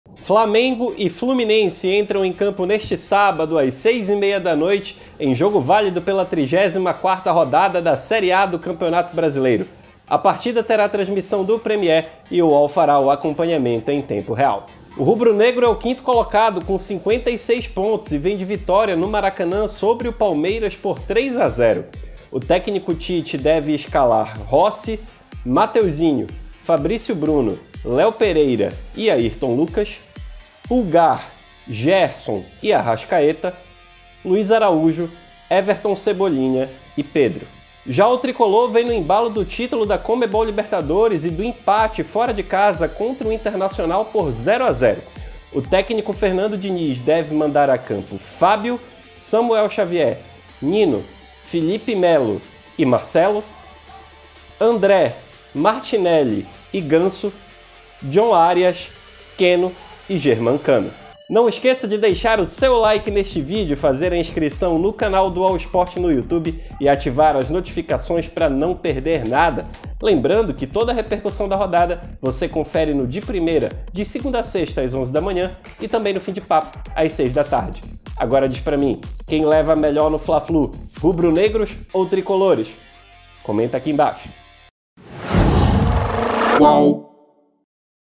FLAMENGO X FLUMINENSE: ONDE ASSISTIR AO VIVO, HORÁRIO E ESCALAÇÕES PELA 34ª RODADA DO BRASILEIRÃO
Flamengo x Fluminense se enfrentam às 18h30 (horário de Brasília) deste sábado (11), no Maracanã, em clássico válido pela 34ª rodada do Campeonato Brasileiro. A transmissão ao vivo será feita com exclusividade pelo Premiere no pay-per-view. O Placar UOL também acompanha todos os lances do Fla-Flu ao vivo e em tempo real.

Confira a provável escalação do Flamengo de Tite, Rossi, Ayrton Lucas, Pulgar, Gerson, Arrascaeta, Luiz Araújo, Éverton Cebolinha, Gabigol e Pedro, e a provável escalação do Fluminense de Fernando Diniz, Fábio, Nino, Felipe Melo, Marcelo, André, Arias, Keno e Cano.